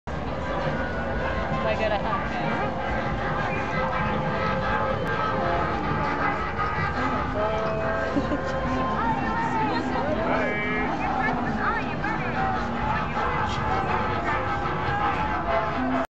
Science tent, Green Futures Field, Glastonbury 2017
Awesome vibrating meditation bowl by the Science Tent, Green Futures Field, Glastonbury 2017